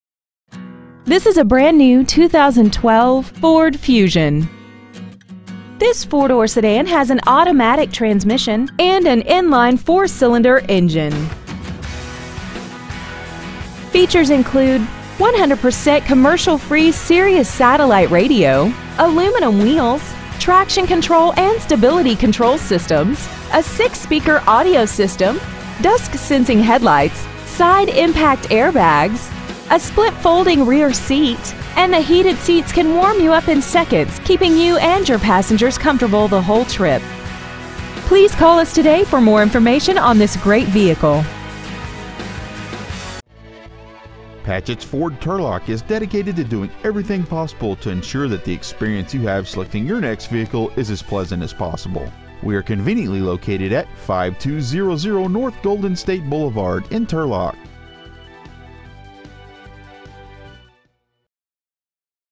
2012 FORD FUSION Turlock CA
Year : 2012
 Make : FORD
 Model : FUSION
 Engine : 2.5L I4 16V MPFI DOHC
 Trans . : 6-SPEED AUTOMATIC
 Exterior : WHITE PLATINUM TRI-COAT METALLIC
 Miles : 10
 Interior : MEDIUM LIGHT STONE
 Stock : 12237

 5200 N Golden State Blvd